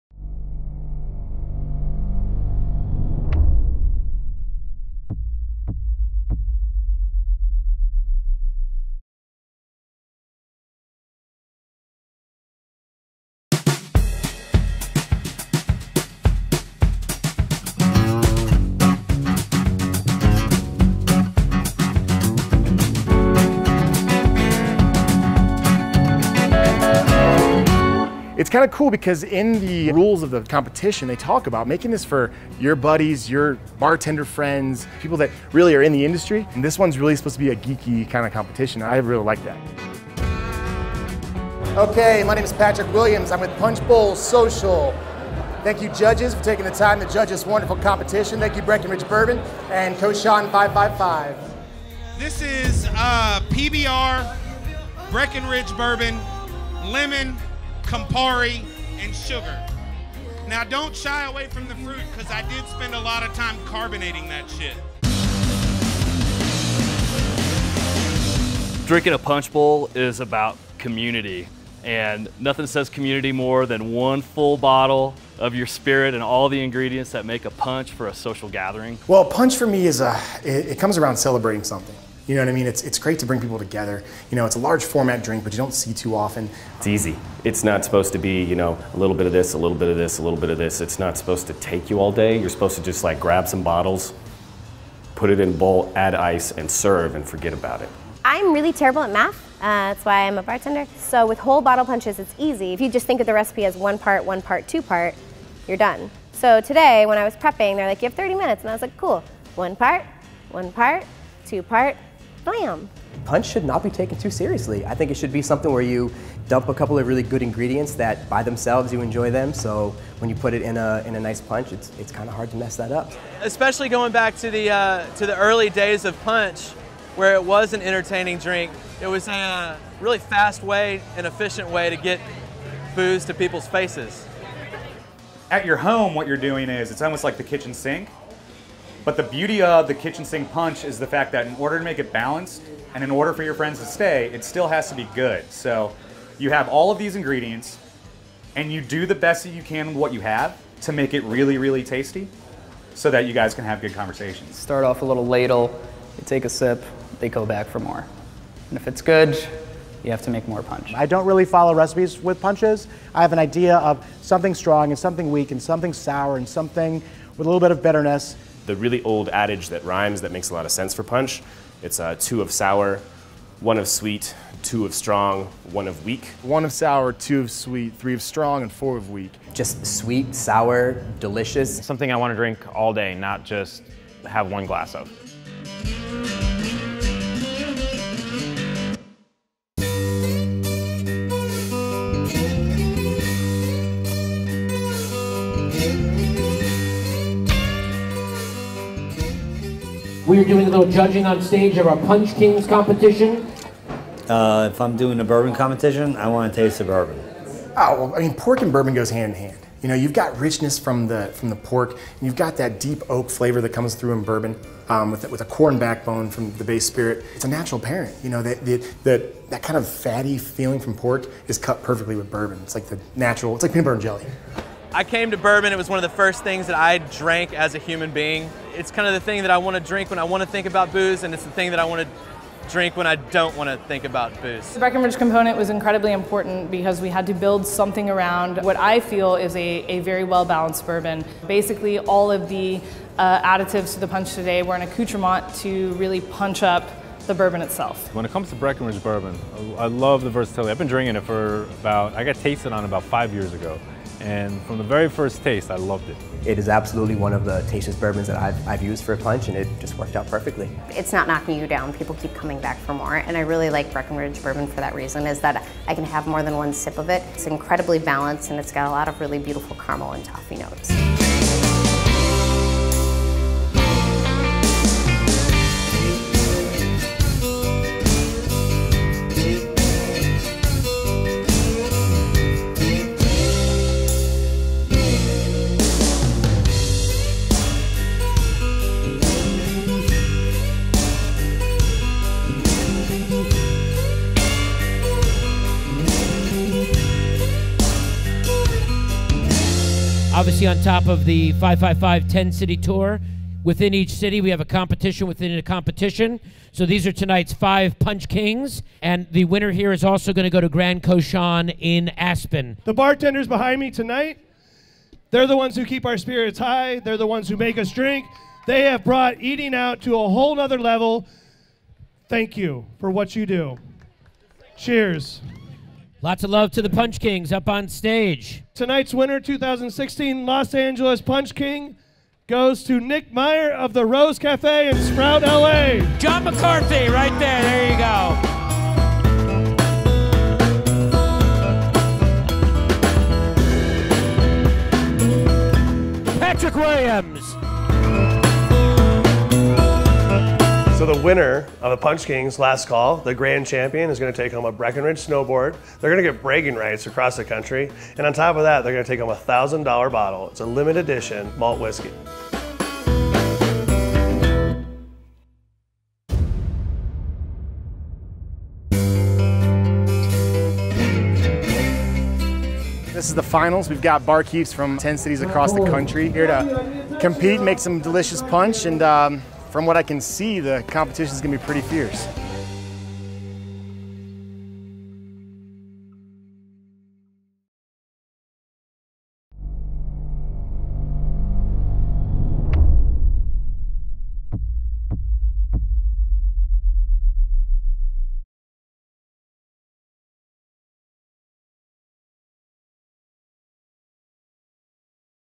Punch Kings Series 01 Episode 02: Whole Bottle
We talked to Bartenders across the country about using a whole bottle of Breckenridge bourbon in their punch and the other special ingredients they add to it.

Punch Kings is a competition within Cochon 555 where participating bartenders are given 30 minutes to prepare 1 punch bowl featuring 1 bottle of spirits along with their special ingredients.